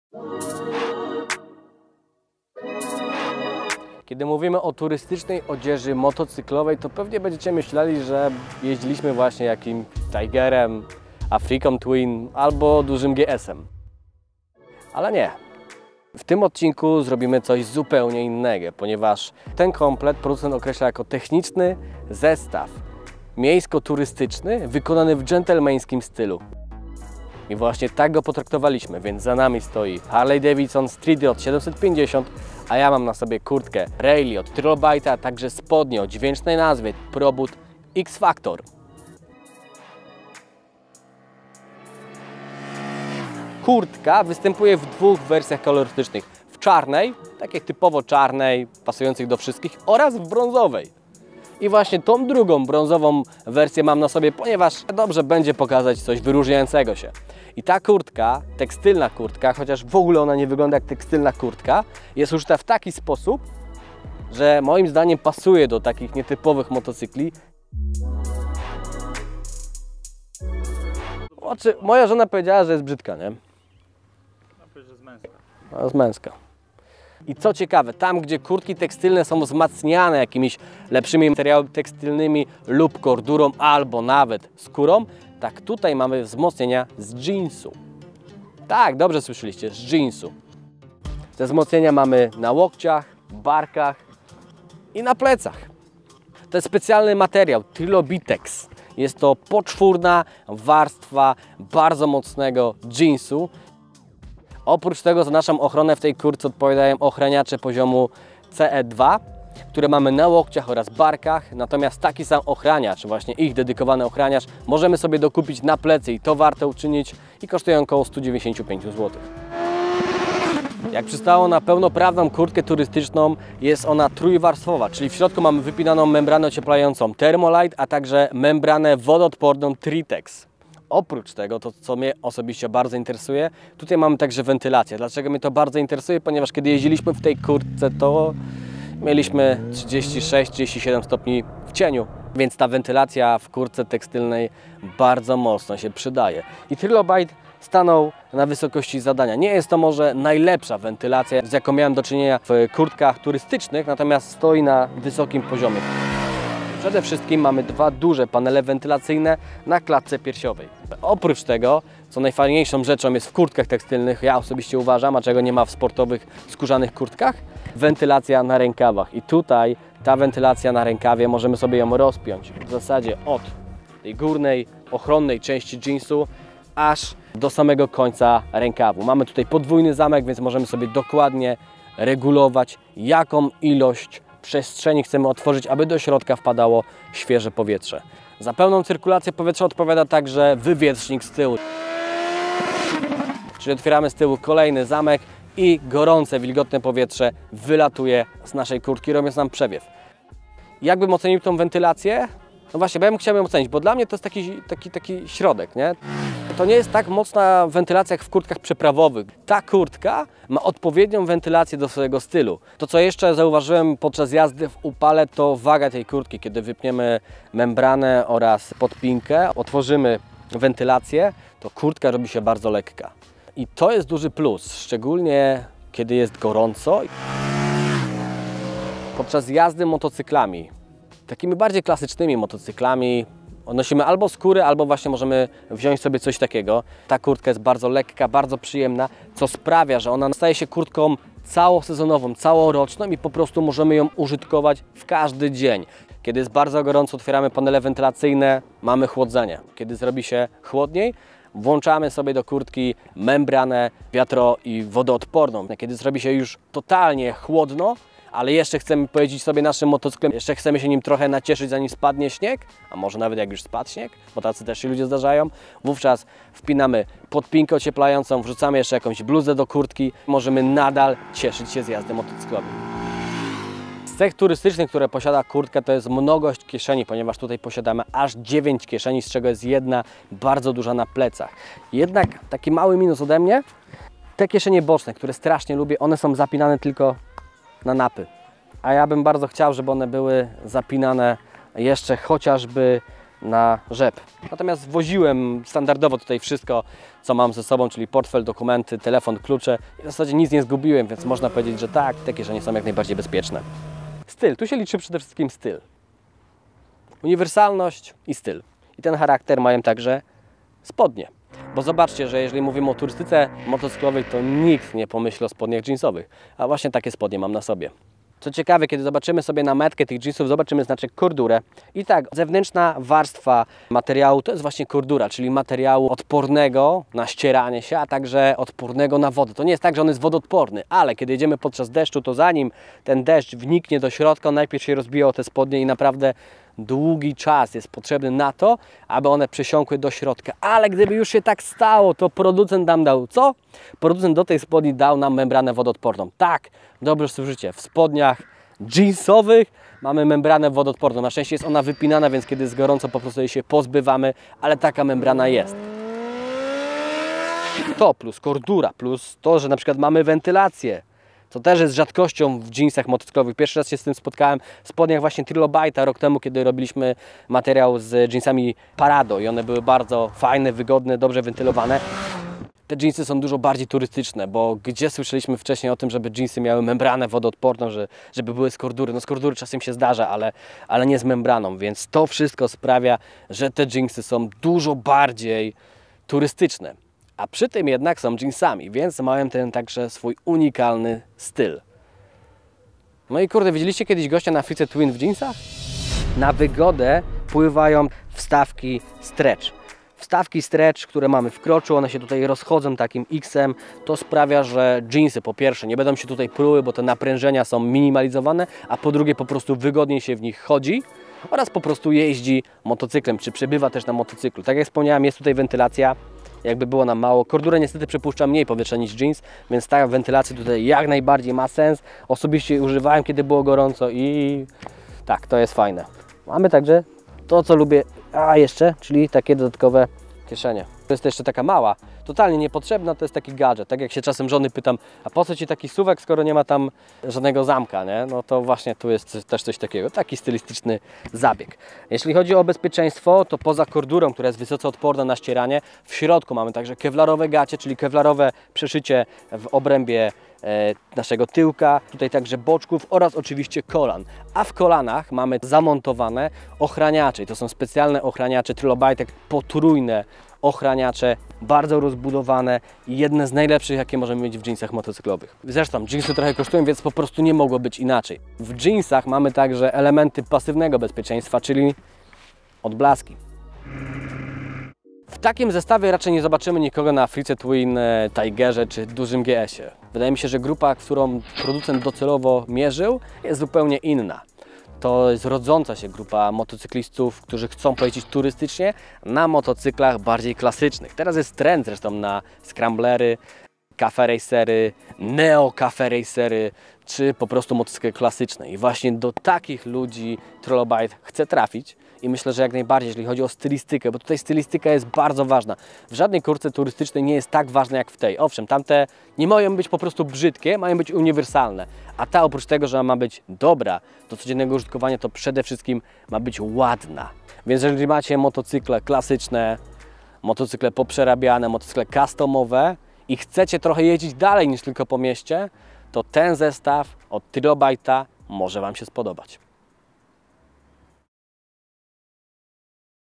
Kurtka Turystyczna Wzmacniana Jeansem? Trilobite 1892 Rally + jeansy Probut X-Factor
Podczas długoterminowego testu Harley-Davidson Street Rod 750 sprawdziliśmy komplet od Trilobite. Na komplet złożyła się oryginalna kurtka Trilobite 1892 Rally, oraz jeansy Trilobite Probut X-Factor 1663. Na czym polega ich nieszablonowość? Zobaczcie materiał!

Trilobite 1892 Rally to kurtka tekstylna, której partie zostały wzmocnione specjalnie spreparowanym denimem. Poducent zastosował tu rozwiązania znane z kurtek turystycznych i zaadaptował je do wymagań turystów motocyklowych, którzy poza praktycznością cenią również unikatowy wygląd. W ciemno-brązowym kolorze ta kurtka przentuje się naprawdę oryginalnie. A jak się sprawdzi na motocyklu?

Trilobite Probut X-Factor 1663 to jeansy motocyklowe stworzone z myślą o długich trasach i zmiennych warunkach atmosferycznych. Membrana wodoodporna w jeansach motocyklowych? Tak - pierwszy raz spotkaliśmy się z takimi spodniami. Dodajmy do tego wierzchnią warstwę z Cordury, wentylację i zaawansowane ochraniacze, a okaże się, że Probut X-Factor to odzież niemal wyczynowa.

Dołącz do nas! Co tydzień nowe video!